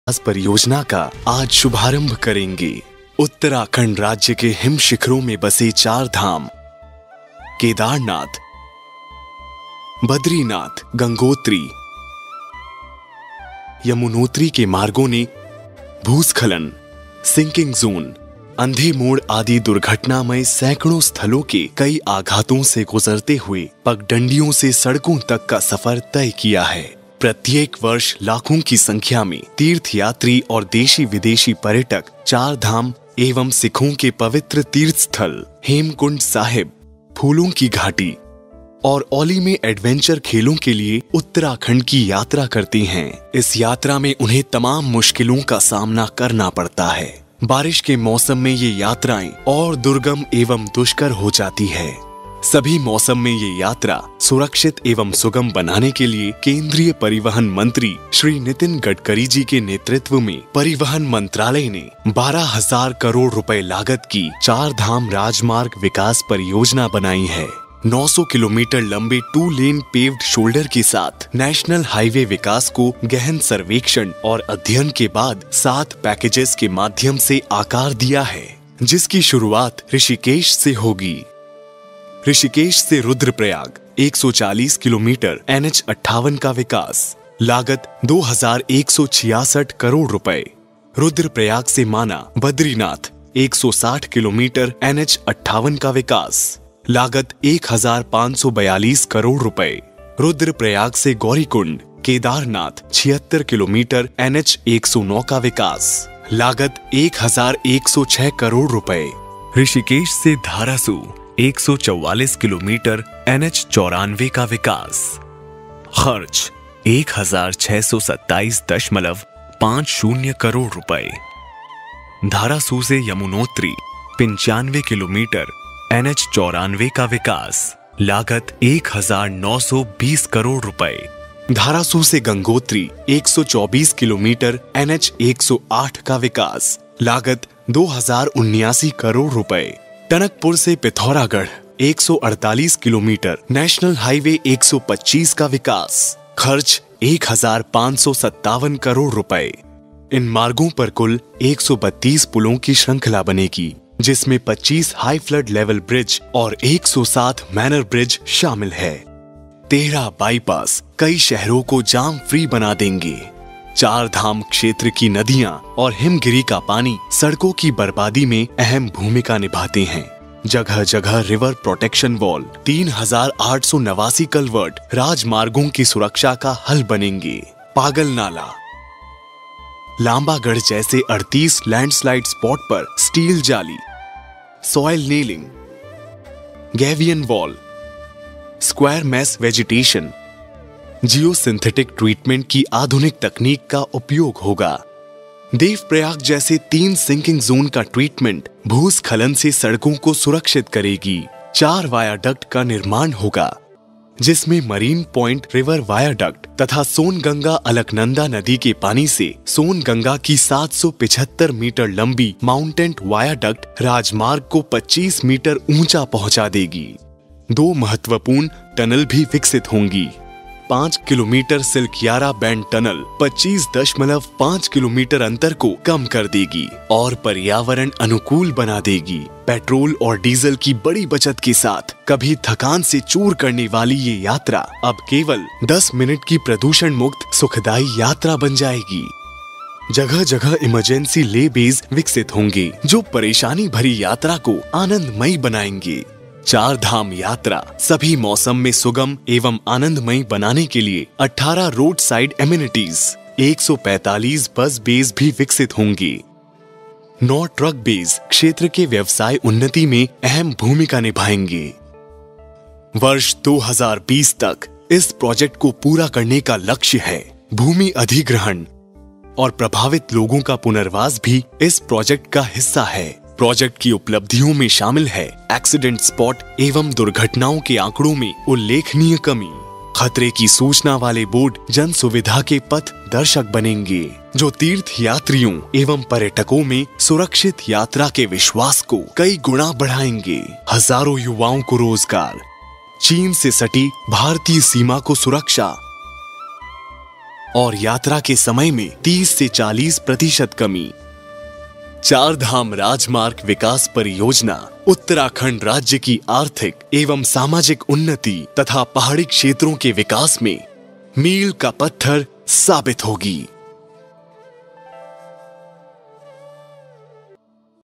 Char Dham Highway Project: All Weather Road Project for Pilgrimage in Uttarakhand
Char Dham, Chardham Project, Char Dham Project Video, Char Dham Highway Project

There are 132 bridges, 13 bypass and 2 tunnels included in Chardham Mahamarg Vikas Pariyojna. Nitin Gadkari and PM Narendra Modi are monitoring the progress of Rs 12000 crores project which will be completed by 2020. It will boost tourists in Uttarakhand state.